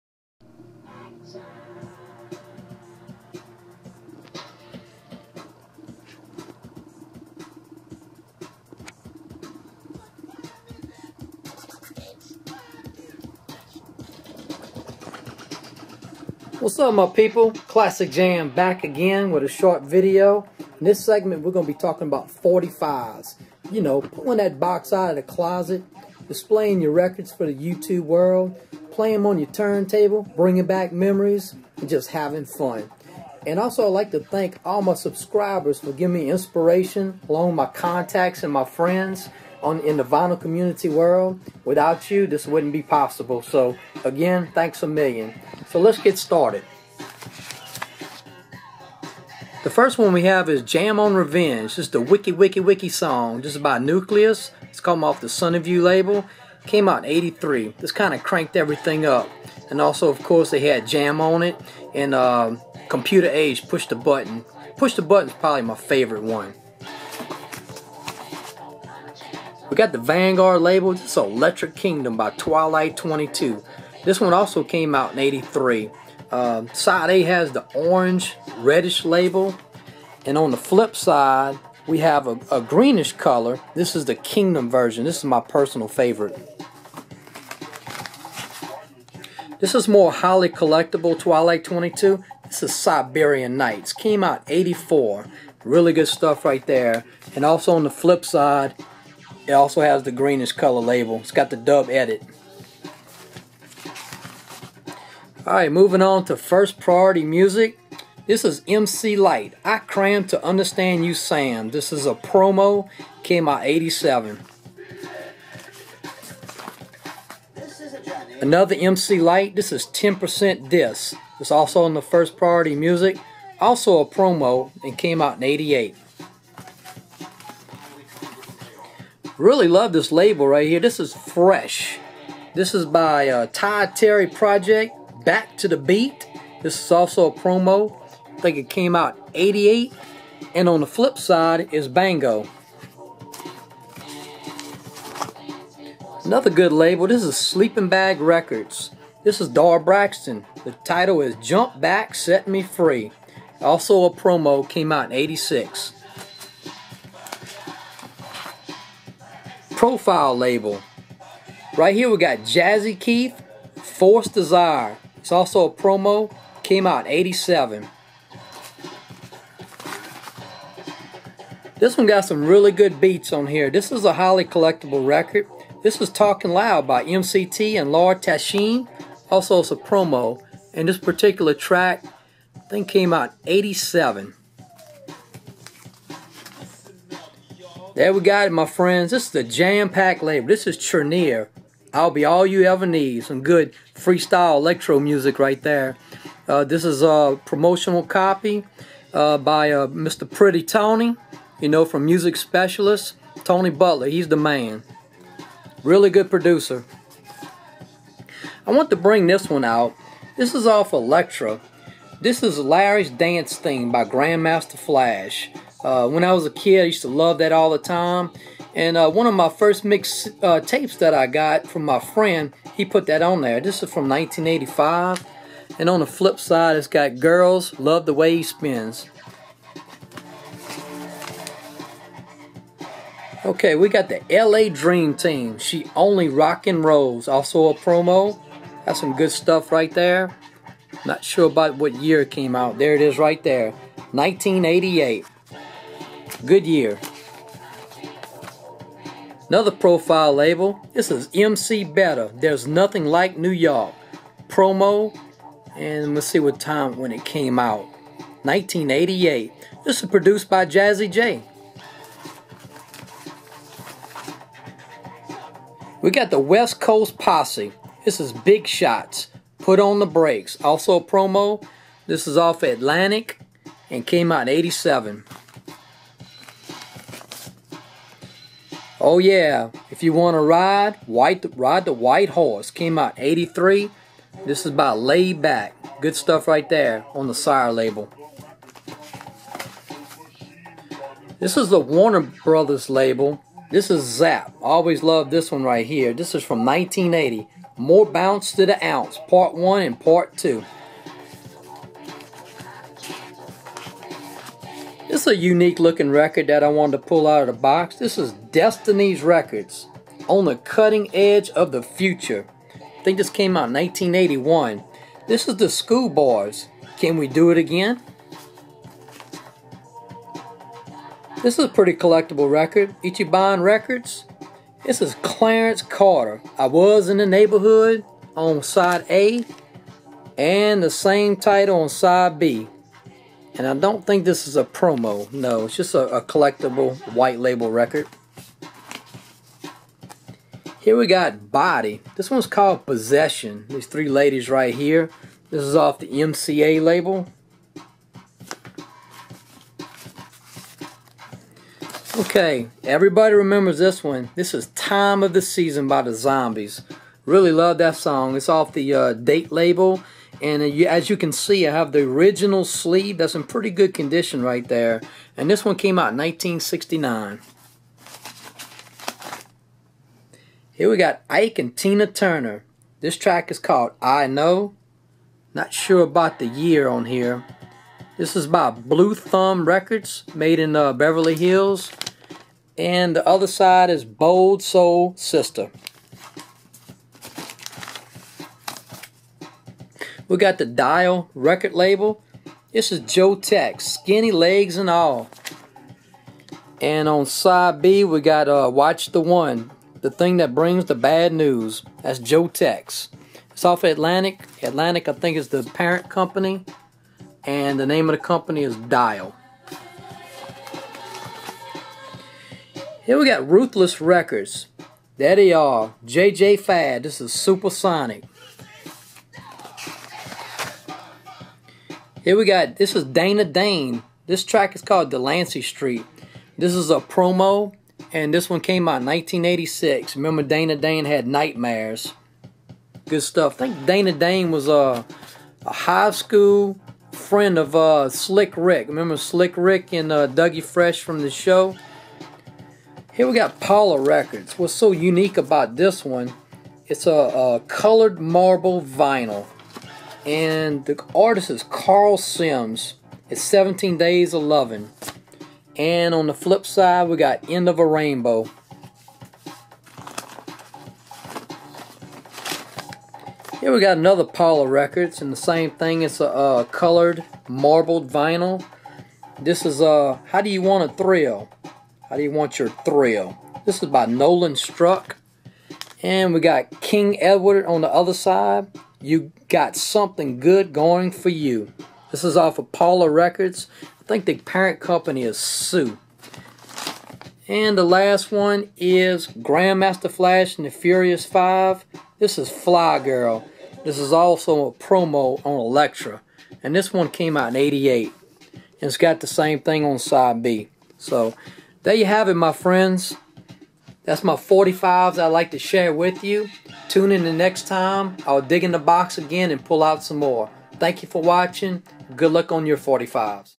Collecting Vinyl - 45's From the Box
Displaying Vinyl 45's (mostly Promos) from the Box stored in the Closet.  Genres include: Electro, Old School Hip Hop, & R&B.  Some Collectable Stuff & Old School Classics.  Encouraging others to share with the Vinyl Community - Post Videos & Have FUN!!